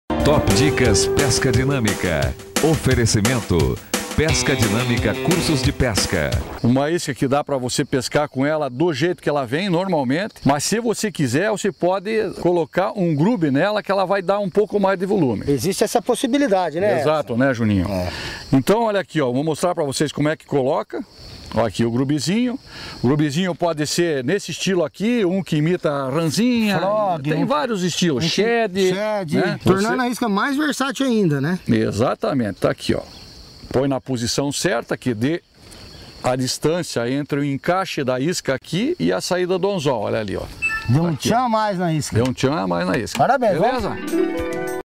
Top Dicas Pesca Dinâmica - Como turbinar a isca Tchau- Tchau para pesca de traíras
Programa Pesca Dinâmica, a melhor alternativa em programas de pesca e dicas para o pescador.